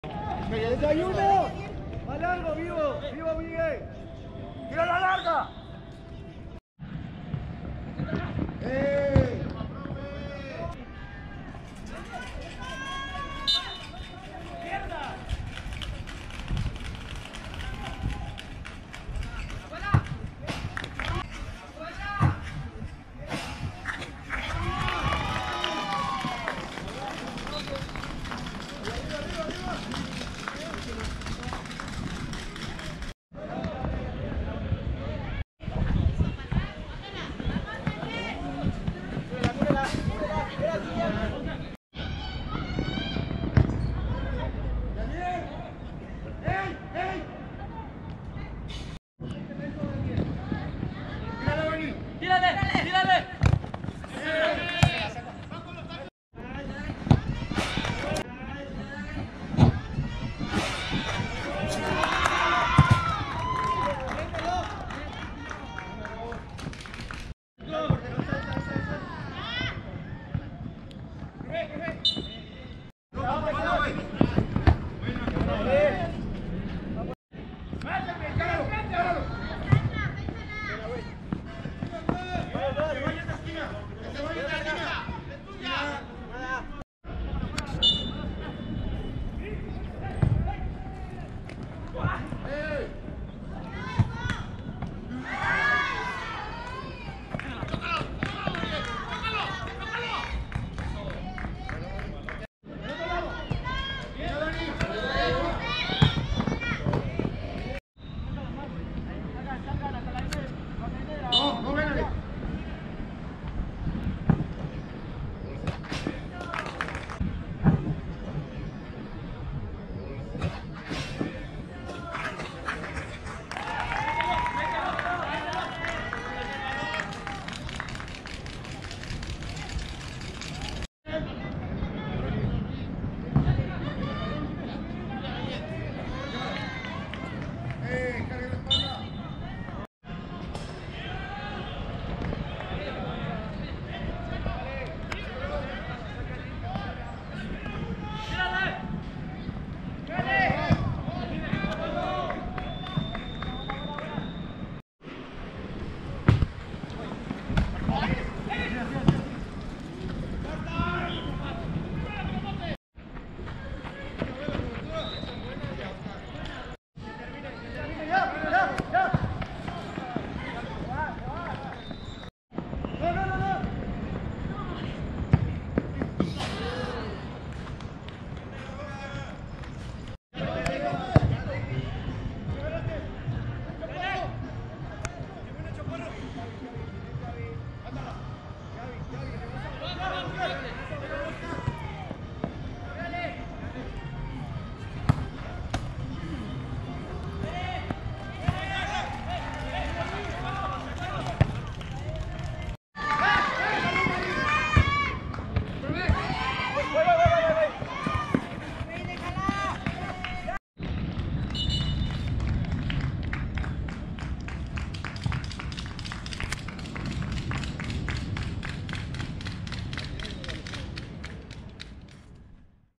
america vs atlas RX SNM | Torneo de Fuerzas Básicas | Highlights
Nuevo torneo de fuerzas básicas con Atlas RX SNM de la jornada 2.

Temporada 1
Capítulo 1

Producción: Ulises Rodríguez